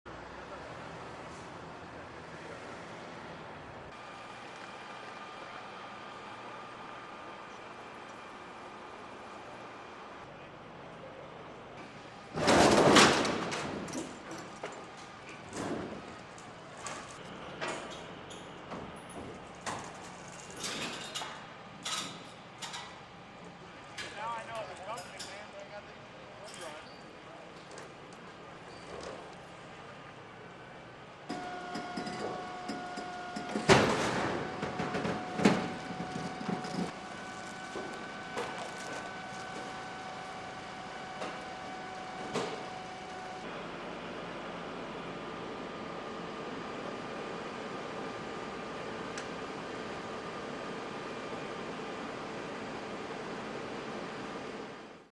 Raw Video: Semi-Truck Gets Stuck Under Bridge
A semi-truck driver in Spokane, Washington, wasn't able to fit his truck under the overpass, pinning it under the bridge. The driver, who has been driving trucks for 30 years, walked away unharmed. (March 4)